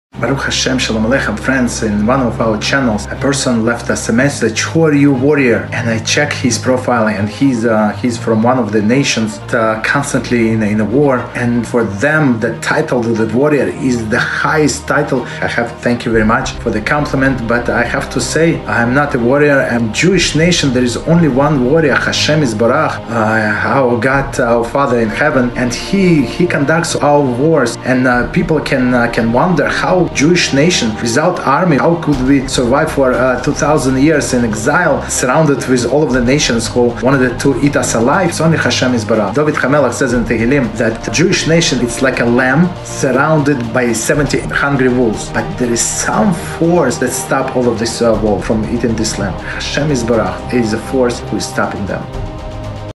Who are you, warrior? ⚔️ Does Israel need an Army❓
When reviewing the history, Jewish people are undoubtedly the most prosecuted nation. With a small population and no army, we survive through the destruction of the Holy Temples, pogroms in England, Spanish inquisition, and Holocaust.
Why did Jews survive?
Because HaShem is the One guiding us, and His covenant with Am Yisrael lasts for eternity. However, if we don't follow His Holy Torah, like a Father berates His beloved son, HaShem punishes us severely to wake us up and follow the right path. Nations are just His staff, our Father in Heaven is the Only One guiding and protecting Am Yisrael.
Wake up, follow the right path. Don't let the horrible history repeats itself. Don't force HaShem punish His beloved children. Join BeEzrat HaShem, learn with Rabbi Lev Leib Lerner, learn Torah, follow the mitzvot, and return to our Father in Heaven.

In this captivating YouTube video, join the viewers as they delve into the thought-provoking words of a wise and compassionate Rabbi. As he opens with the traditional Jewish greeting, "Shalom Aleichem," the Rabbi embarks on a profound reflection prompted by a message left in one of their channels.

The message, "Who are you, warrior?" intrigues the Rabbi, and after investigating the sender's profile, he discovers they hail from a nation perpetually engulfed in conflict, where the title of warrior holds the utmost prestige. With humility and gratitude, the Rabbi acknowledges the compliment but quickly clarifies that he is not a warrior himself.

He proceeds to reveal a profound truth embedded deep in the Jewish faith—one that has allowed the Jewish nation to persevere for 2,000 years in exile despite being surrounded by nations seeking their demise. In the Rabbi's eyes, there is only one true warrior, and that is Hashem YitBarach—their God, their Father in heaven.

Drawing inspiration from the words of King David in Tehillim, the Rabbi beautifully likens the Jewish nation to a gentle lamb encircled by 70 ravenous wolves. Despite the perilous surroundings, the lamb remains unscathed, protected by an inexplicable force—Hashem Yitbarach.

Throughout this enlightening discourse, the Rabbi skillfully elucidates how the Jewish nation doesn't rely on an army or conventional means of defense. Instead, they put their faith in Hashem, believing that He conducts their wars and safeguards them from harm.

With eloquence and unwavering faith, the Rabbi reminds us all that even in the face of seemingly insurmountable challenges, it is trust in a higher power that sustains and shields them from harm. Join the Rabbi on this spiritual journey as he imparts timeless wisdom and encourages viewers to embrace hope, faith, and the power of divine protection. Discover the strength in vulnerability and the peace that comes from surrendering to a force greater than ourselves.

Don't miss this uplifting and inspiring message as the Rabbi's words resonate beyond the screen, touching the hearts and minds of all who seek solace in turbulent times.